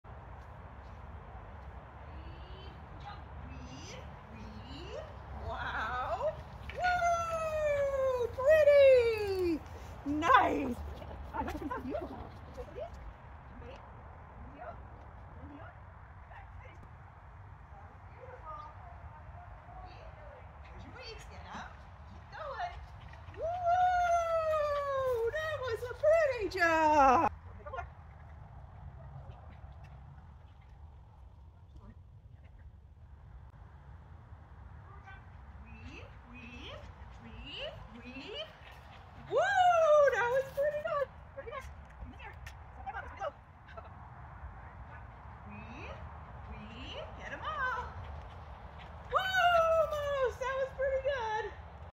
Kaz Agility Weave Training
After the trial I thought we would do a little weaving. He does great when he's on my right. When he's on my left he skipped a bit. But he's doing really well!